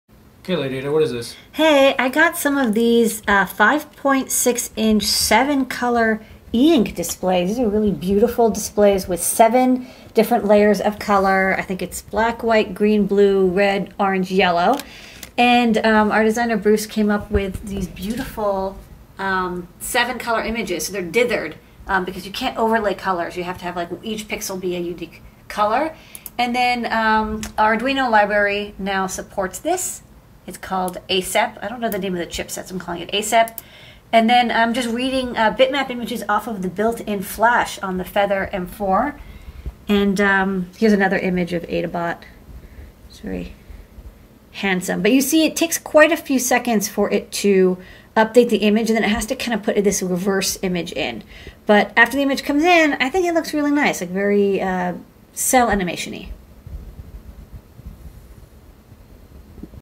7-Color E-Ink display Arduino library works!
This E-Ink display has 7 colors (white, black, red, green, blue, orange and yellow) in a lovely pastel shade. We finally got around to adding support in the Adafruit_EPD library - you just need a microcontroller with 133KB+ of RAM, like this Feather M4! I've got some images on the built-in QSPI flash and writing them to the display. You need to do a 'clean' cycle between draws which makes the EPD a little faint looking but apparently does a good job of de-ghosting. It's really a lovely display and we're looking forward to turning this into FeatherWing or MagTag!